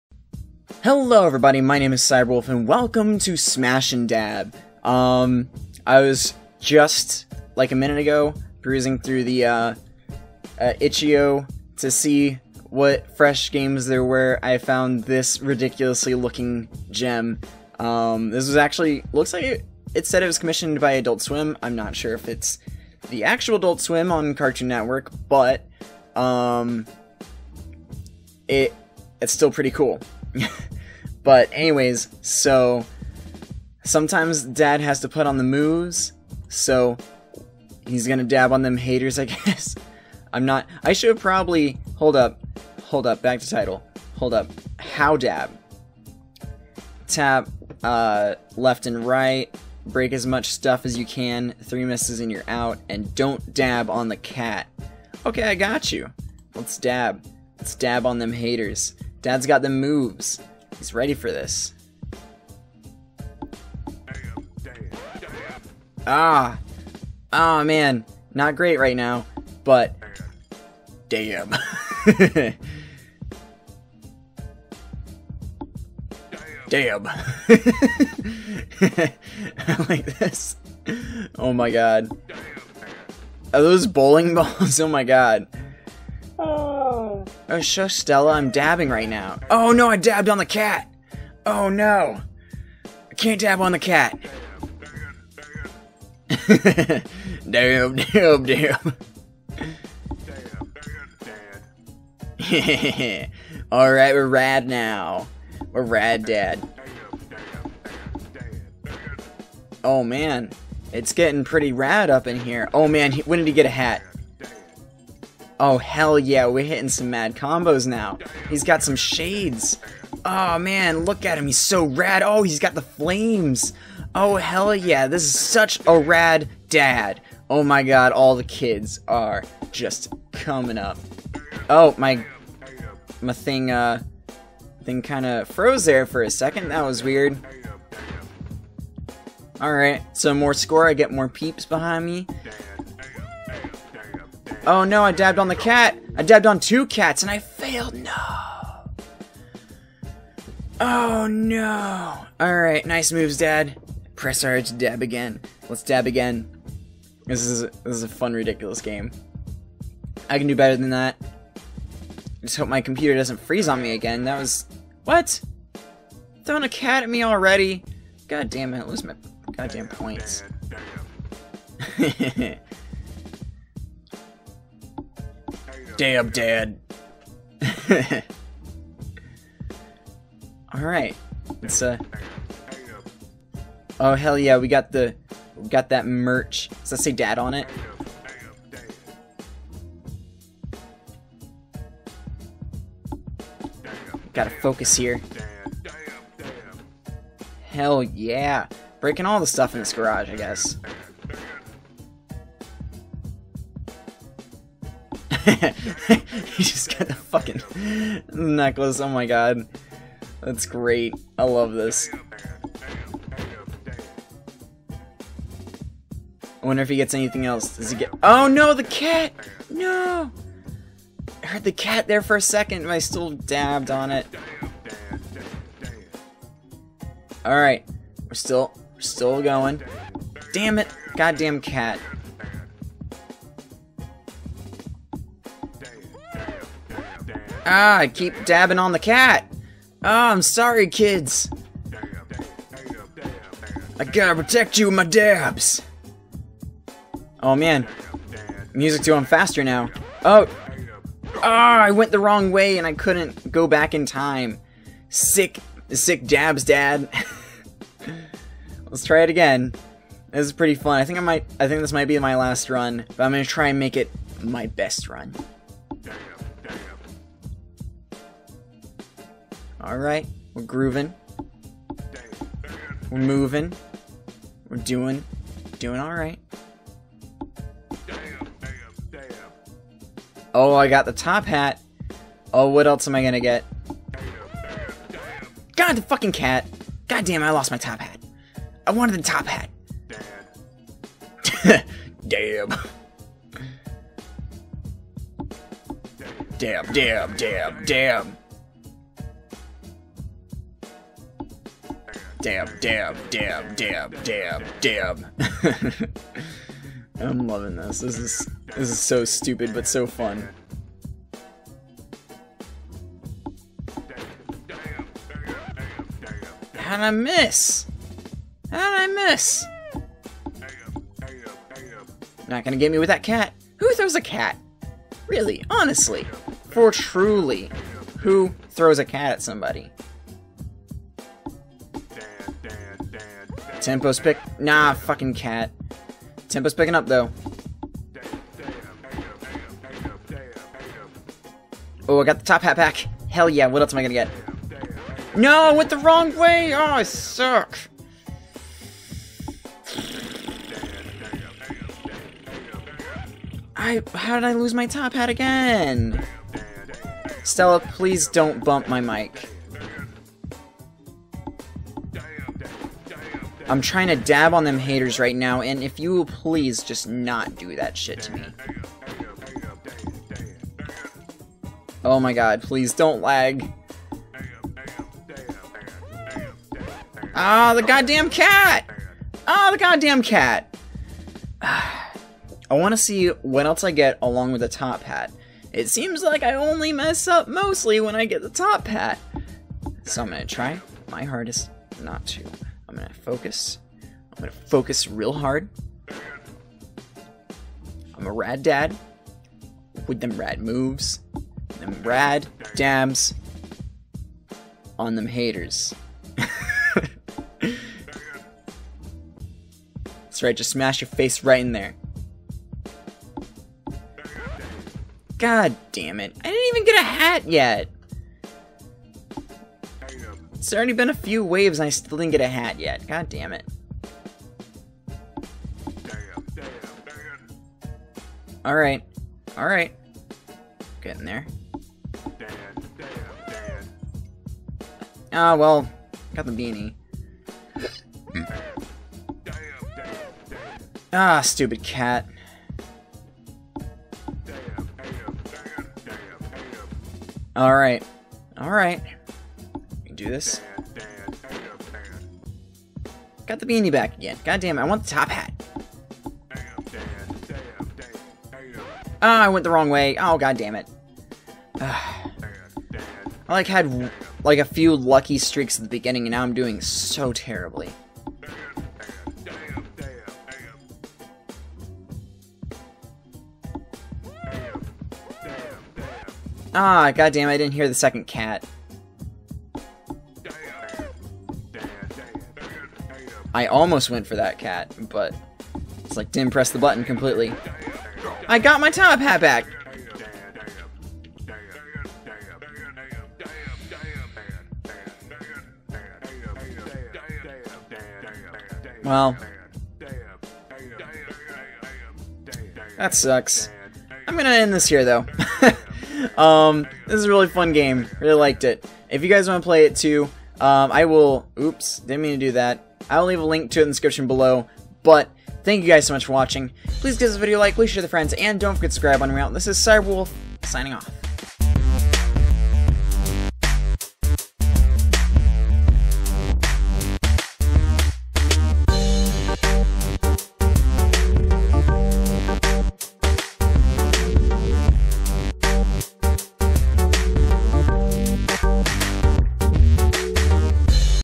Dab On Them Haters, Dad! | Smash N Dab
Dad's got some mad moves and he's gonna prove it!